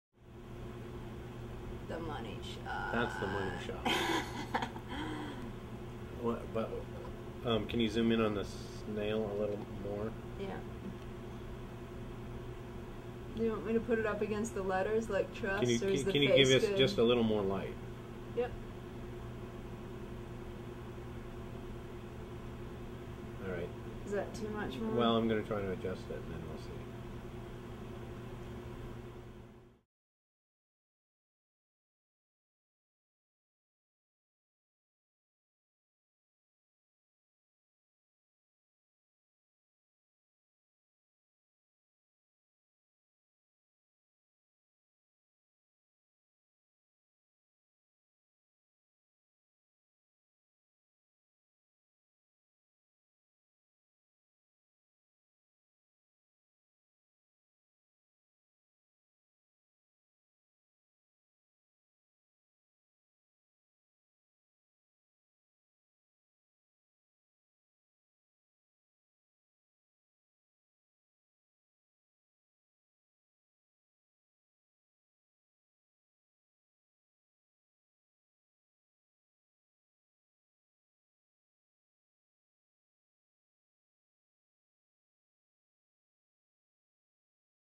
Edwards Aquifer Aquatic Troglobitic Cave Snail
This is a rarely seen and probably never before filmed living, species of cave snail from the Edwards Aquifer.